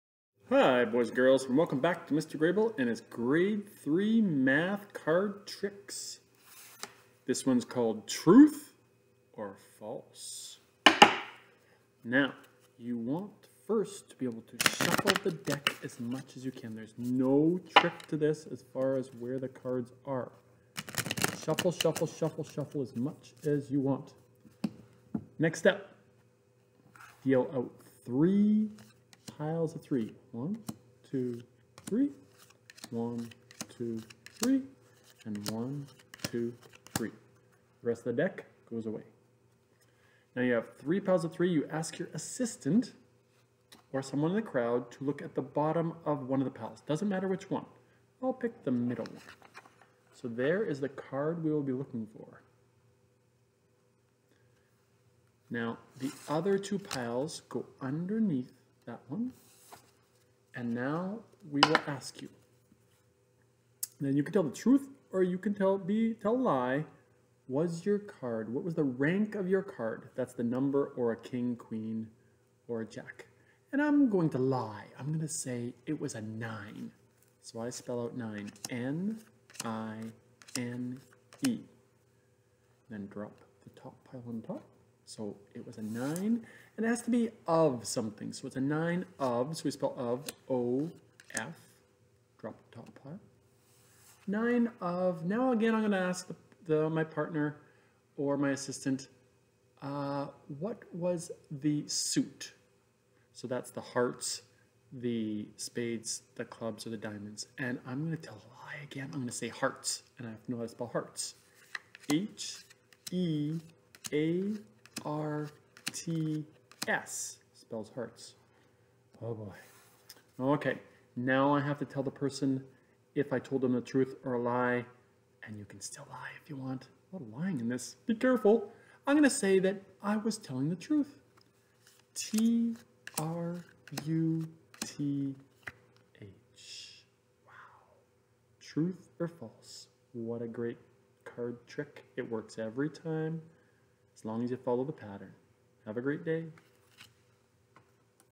Truth or False Card Trick!
This card trick uses mathematical ideas to always have your card come up after your SPELL the Rank(number or King, Queen, Jack) then Spell "of" and then Spell the Suit(clubs, spades, hearts, or diamonds), and finally Spell TRUTH or FALSE OR any 5 letter word/name!